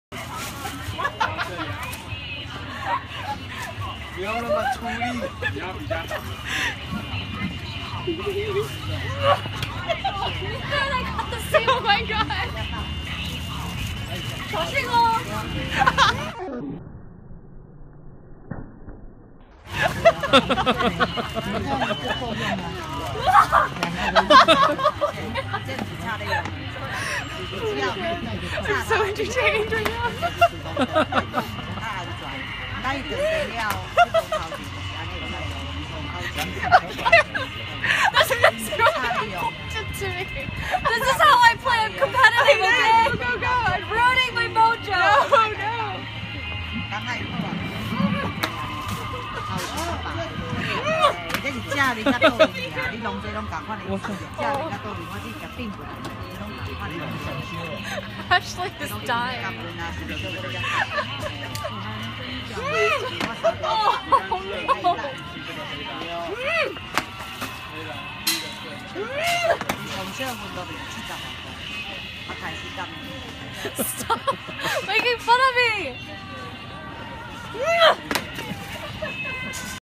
Throwing darts at Bangka night market in Taiwan
Throwing darts against balloons at the Bangka night market